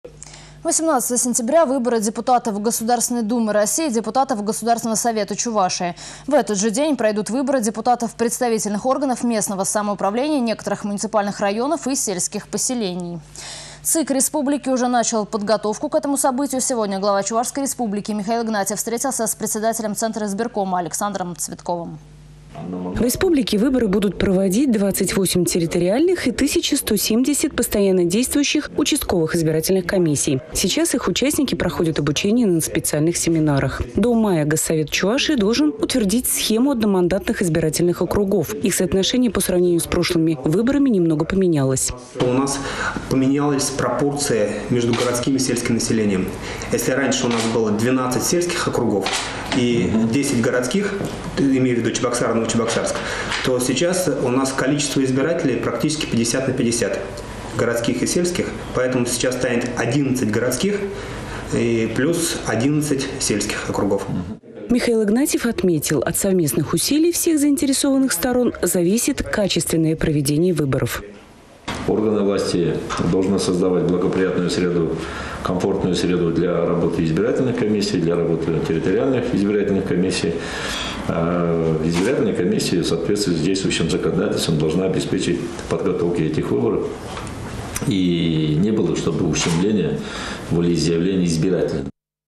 Михаил Игнатьев встретился с Александром Цветковым.
18 сентября - выборы депутатов Государственной Думы России  и депутатов Государственного Совета Чувашии. В этот же день пройдут  выборы депутатов представительных органов местного самоуправления некоторых муниципальных районов и сельских поселений. ЦИК республики уже начал подготовку к этому событию. Сегодня  Глава Чувашской Республики Михаил Игнатьев встретился с председателем Центризбиркома Александром Цветковым.